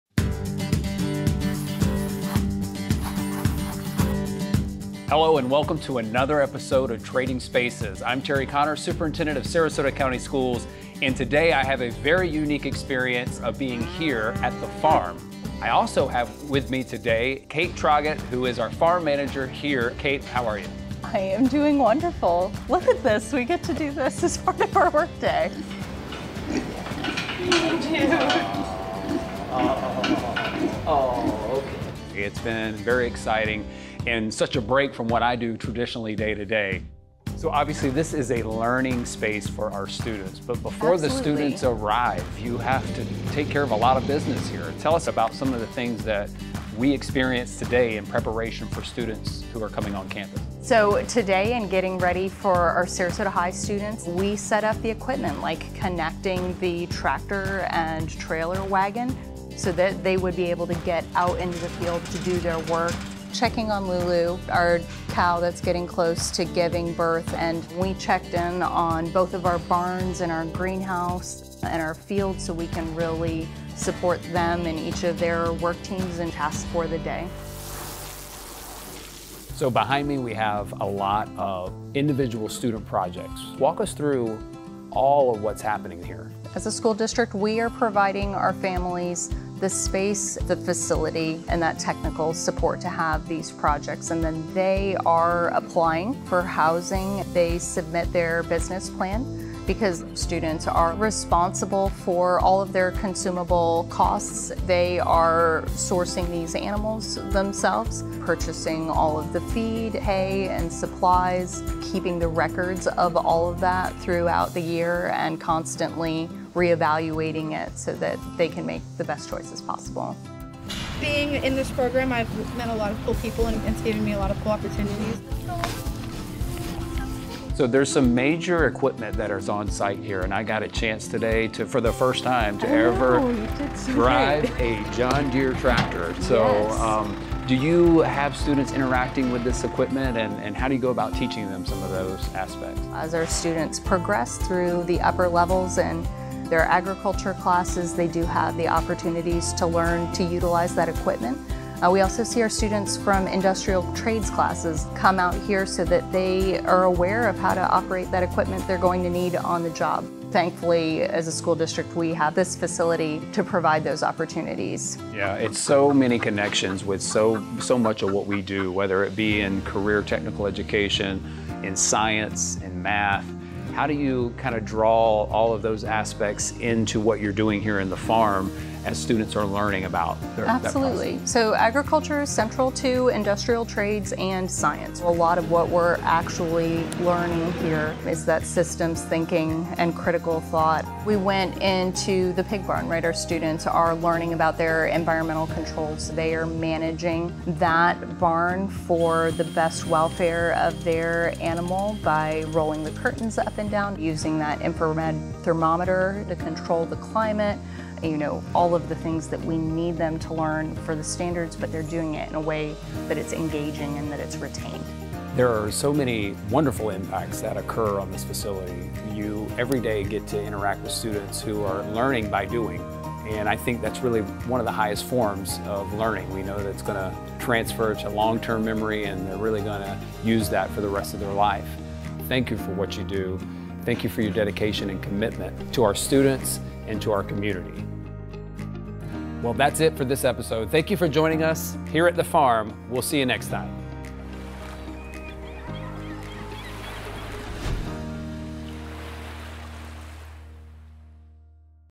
Trading Spaces with Terry Connor | Farm Manager
In this episode, Superintendent Terry Connor steps into the role of Farm Manager to experience hands-on learning at our district farm. From caring for animals to preparing equipment and supporting student-led projects, this space shows how agriculture connects to science, career and technical education, and real-world skills. 🌱🚜